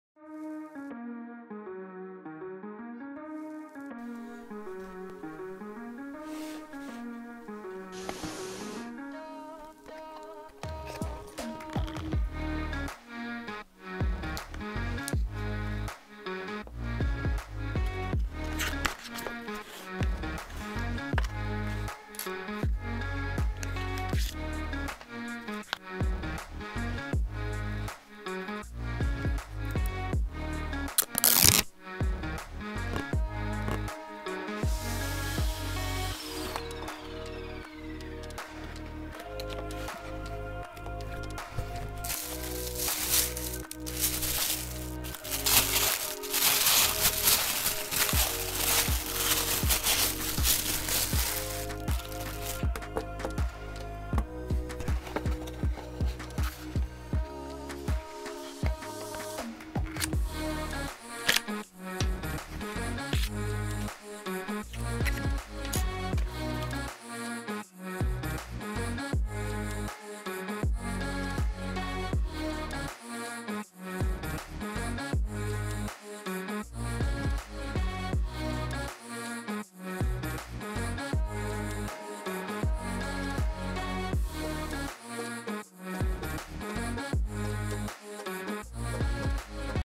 Unboxing Logitech MX Series | Mac Keyboard & Mouse | Episode #4
Logitech has always been one of the best brand on building computer accessories. Specially the MX Master Series gives you that professional touch to your work desk.
I bought these from their official website including a student discount but you can also find them easily on amazon.